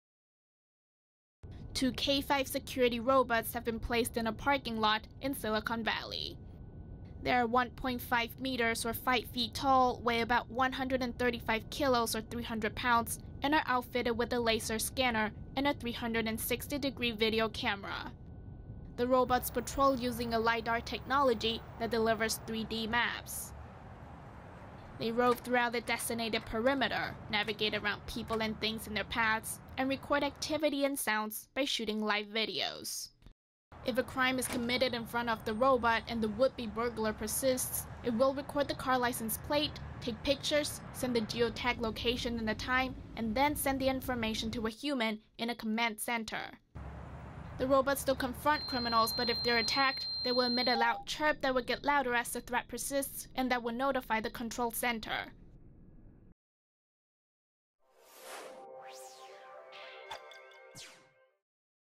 K5 crime-fighting robots go on patrol in Silicon Valley
Two K5 security robots have been placed on patrol in a parking lot in Silicon Valley.

They are 1.5 meters tall (or 5 feet), weigh about 135 kilos (or 300 pounds), and are outfitted with a laser scanner and a 360-degree video camera, CBS News reported.

The robots rove throughout a designated perimeter, navigate around people and things in their paths, and record activity and sounds by shooting live videos.

They also use a LIDAR technology that delivers 3D maps.

If a crime is committed in front of the robot and the would-be burglar persists, it will record the car license plate, take pictures, send the geotag location and the time and send everything to a human in the command center.

The robots don’t confront criminals, but if it attacked, will let out a chirp that will get louder and louder as the threat persists and notifying the control center.

According to CBS News, Knightscope, the company that developed the robots, said there is a waiting list of about four dozen companies in line to acquire a K5.

The companies plan to use them next year in outdoor settings such as college campuses or open air malls.